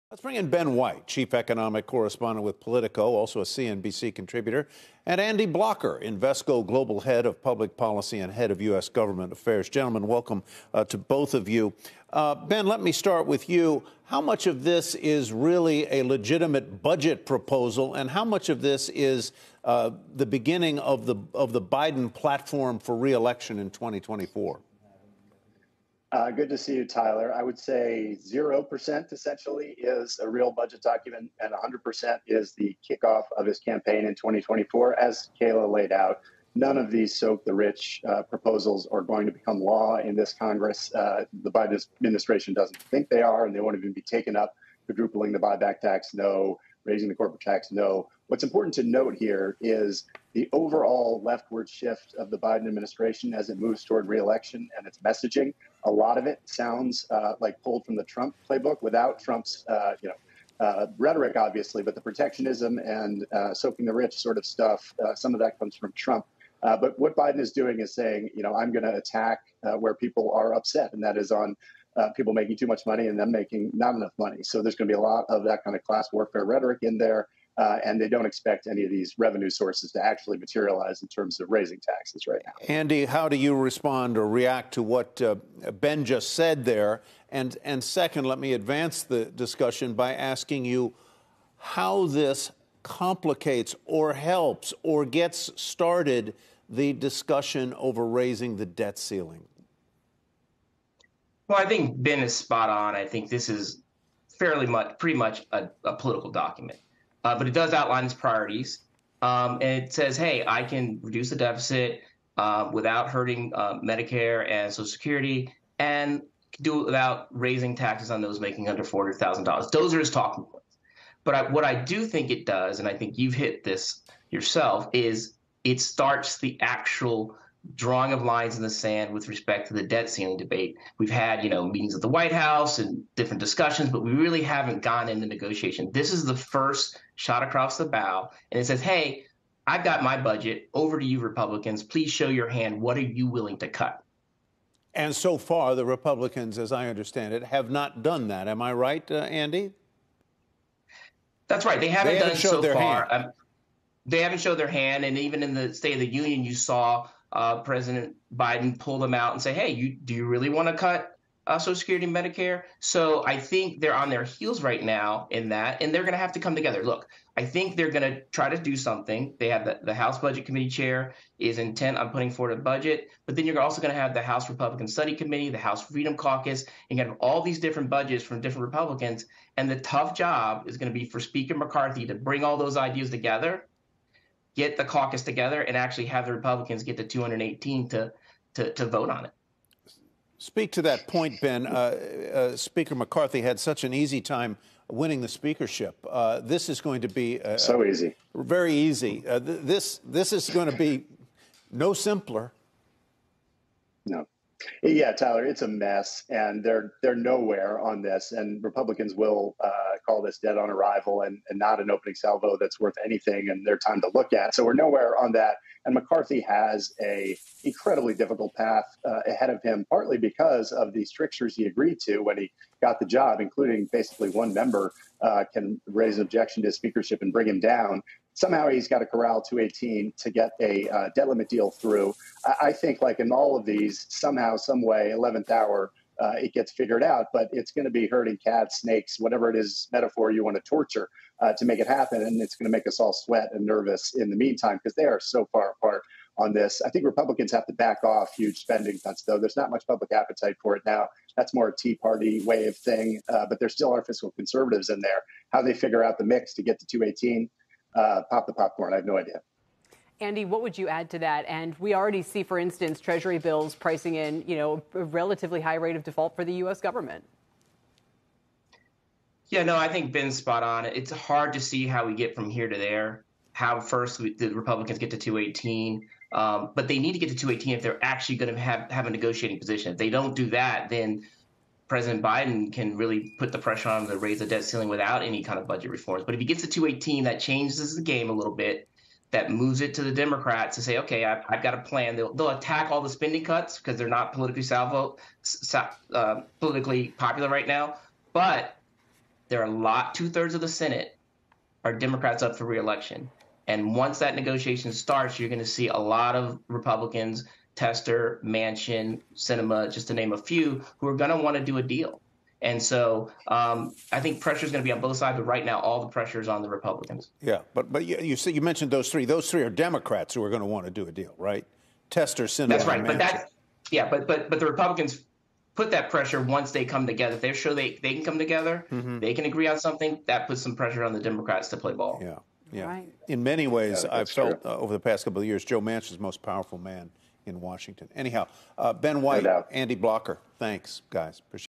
Public appetite for Republican spending cut proposals wanes
Ben White, chief economic correspondent with Politico, and Andy Blocker, Invesco global head of public policy and U.S. government affairs, joins 'Power Lunch' to discuss Biden's budget proposal and lay the groundwork for election season.